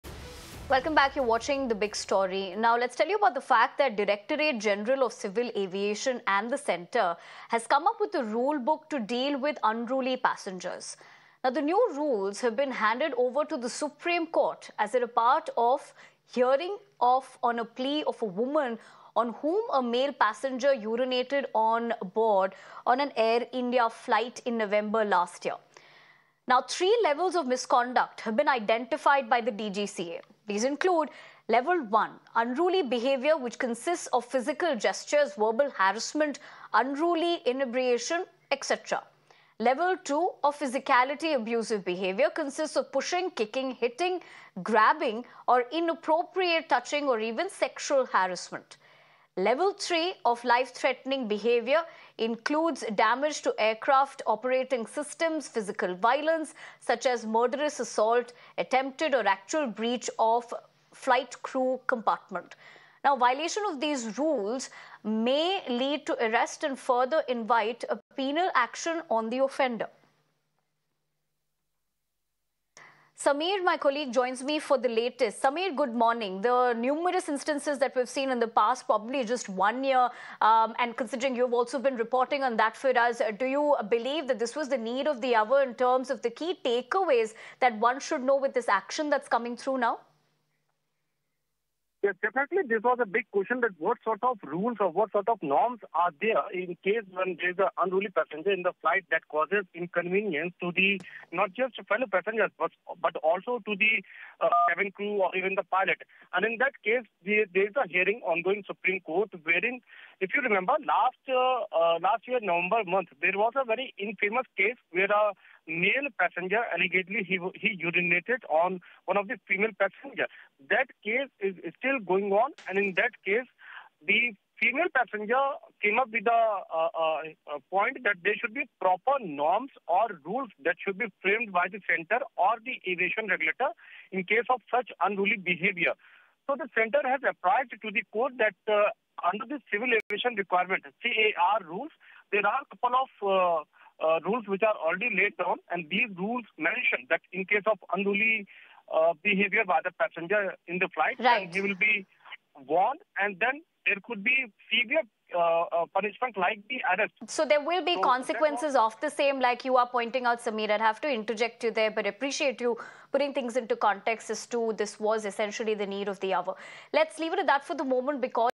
Centre Implements Crackdown On Mid-Air Misconduct: DGCA Issues Rule Book To Supreme Court | Top News
Centre implements crackdown on mid-air misconduct with three levels of designated misbehavior. DGCA issues rule book to supreme court, warning of arrests and penal actions. Zero tolerance policy enforced, no tolerance for touching or harassment. Aviation authorities aim to ensure a safe and secure air travel environment.